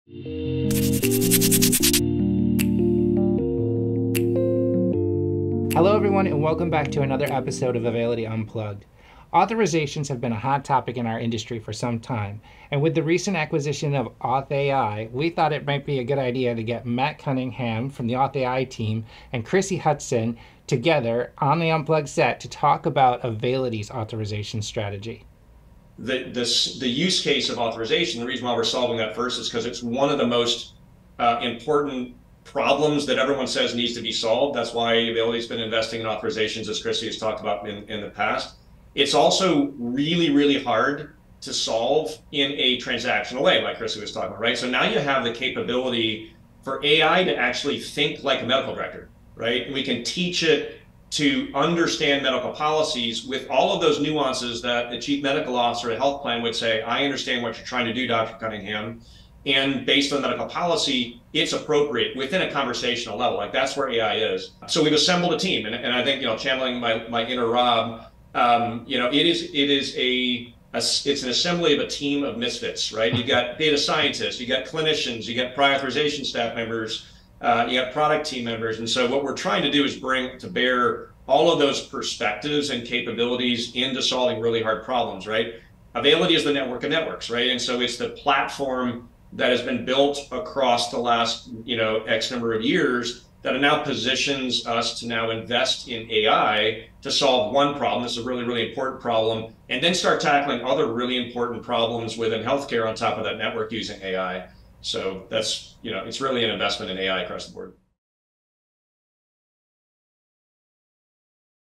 Unleashing Innovation: Authorizations as a Use Case for Artificial Intelligence (AI
We're excited to bring Krisi Hutson, Senior Director of Product Development, and Matt Cunningham, Executive Vice President of Product at Availity, together on the Unplugged set to delve into a subject that’s been creating a lot of buzz - Authorizations! From defining key terms like “Utilization Management” and “Prior Authorizations," to exploring how Availity AuthAI™, delivers speed and accuracy to medical necessity reviews, it's an episode you won't want to miss!

To learn more about Availity AuthAI or to schedule a meeting with the team,